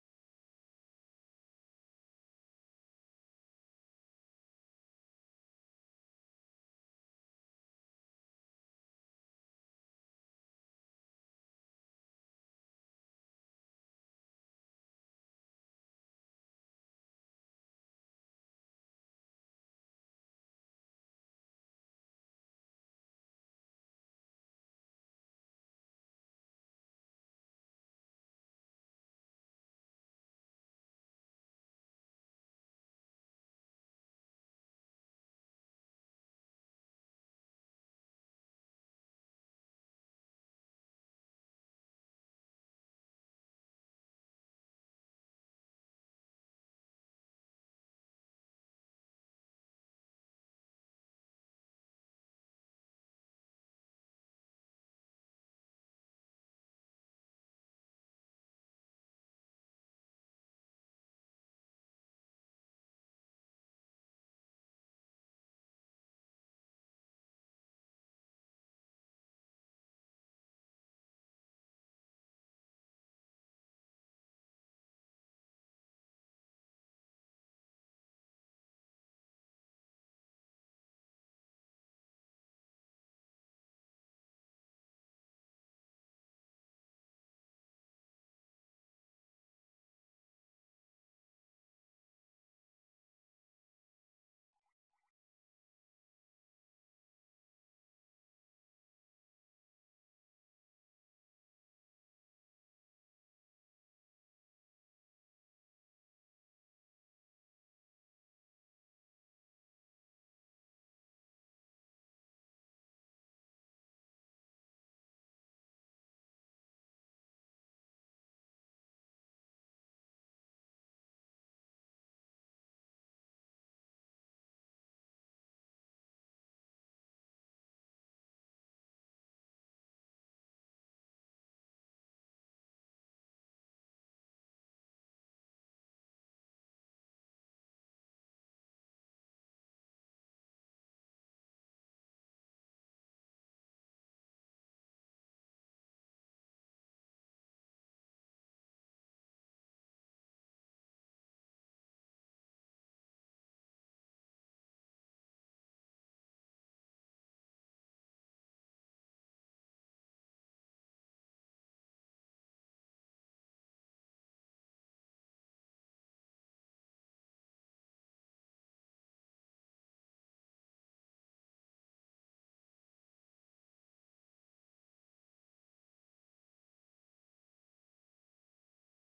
RHEL 7 Configure systems to launch virtual machines at boot
This video covers following  topics of RHCSA Series:

Topics Covered:
Configure systems to launch virtual machines at boot